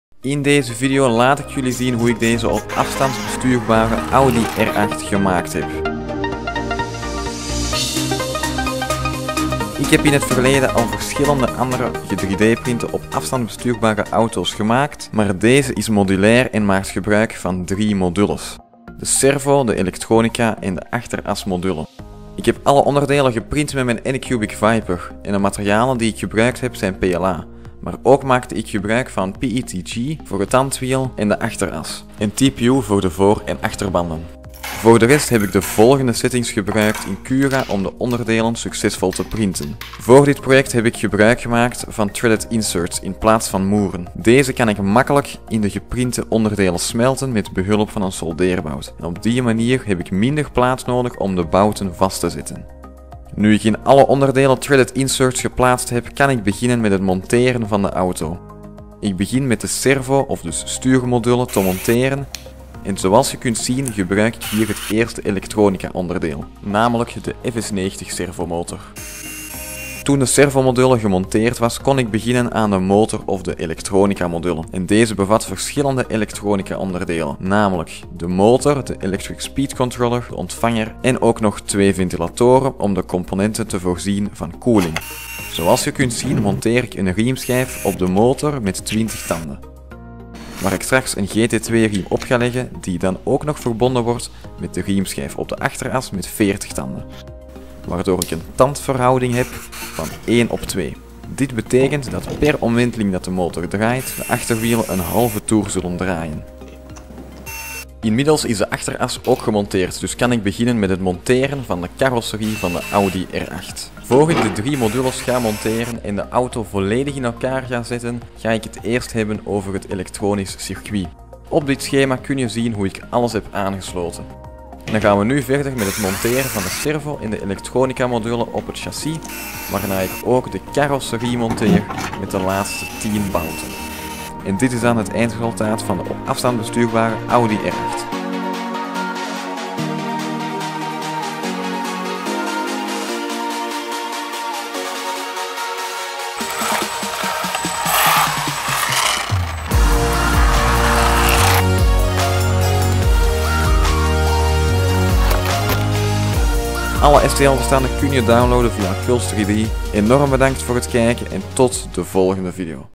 DIY Fully 3D printed 1:18 Audi R8 - OpenRC Car V4
I designed this awesome Audi R8 which is a remote controlled (RC) car! The topspeed of the car is 38,49 km/h!
The design is modular so in the future I can change the chassis only a little to add other carbodies or to design other modules for steering, electronics and driving!

- (2x) M3 Washer
- (2x) M8 Washer
- (1x) GT2 timing Belt - 6mm - closed 158 mm loop:
- (1x) GT2 Pulley - 6 mm belt - 40 teeth - 8 mm axis:
- (1x) M2 Nut
- (57x) M3 Short Threaded Inserts: Turmberg3D - draadinzetstuk assortiment kort M2.5 / M3 / M4 / M5 (50/50/50/50 stuks) (assortimentsbox kort):
- (1x) Transmitter, Receiver, Brushless motor, ESC
- Only motor and ESC
- (1x) 7,4V Li-ion battery
- (1x) T-plug connector: